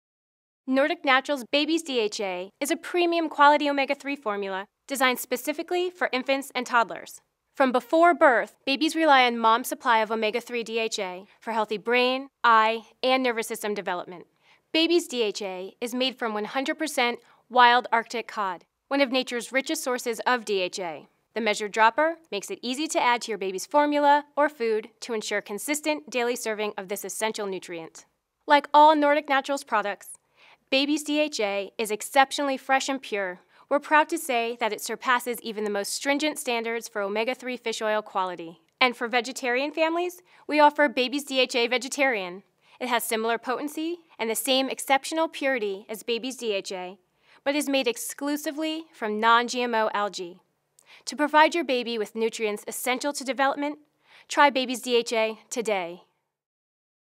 Nordic Naturals Baby's DHA with Vitamin D3 and Vegetarian Formulas
Most children are deficient in EPA and DHA, the omega-3 essential fatty acids in fish oil. These vital nutrients are crucial building blocks of child development and play key roles in every aspect of health, including brain and eye function.*

Supplementing with Nordic Naturals fish oil is a safe, pure, and effective way to give your child's mental and physical well-being a nutritional head start.* Nordic's tasty, patented, and natural fruit flavors please even the pickiest palates and leave many kids asking for more!

Baby's DHA Vegetarian
Made exclusively from microalgae, nature's original source of marine omega-3s. Nordic Naturals Baby's DHA Vegetarian is formulated for the specific omega-3 nutritional needs of infants, and is a safe, effective way to ensure that babies 6 months or older (or as directed by your pediatrician) get the DHA they need for healthy development.*
-Great for babies 6 months or older
-100% vegetarian source of marine omega-3s
-Measured dropper for easy addition to formula or food
-Supports healthy brain, vision, and nervous system function*
-Non-GMO and hexane free
-Certified by the America Vegetarian Association

Baby's DHA with Vitamin D3
Made exclusively from our Gold Standard Arctic Cod Liver Oil™, Nordic Naturals Baby's DHA is a safe and effective way to ensure that growing babies get the DHA they need for healthy development.* Like all Nordic Naturals fish oils, Baby's DHA is third-party tested and surpasses strict international standards for purity and freshness.
-Great for babies 5--35 lbs
-Made from 100% wild Arctic cod
-Supports healthy brain, eye, and nervous system function*